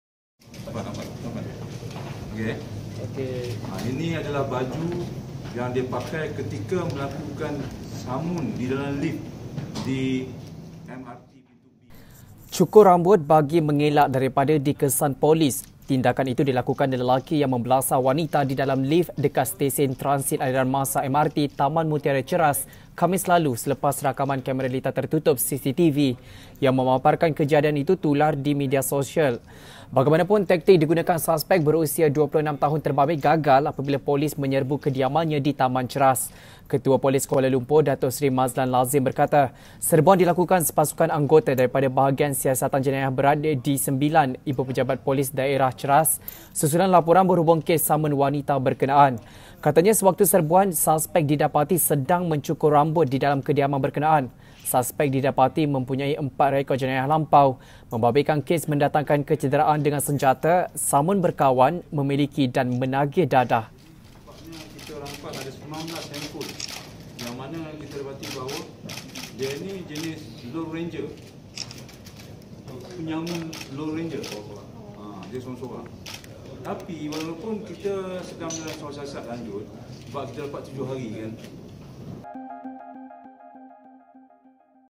Botak pun dapat cam jugak
CUKUR rambut bagi mengelak daripada dikesan polis.

Bagaimanapun, taktik digunakan suspek berusia 26 tahun terbabit gagal apabila polis menyerbu kediamannya di Taman Cheras.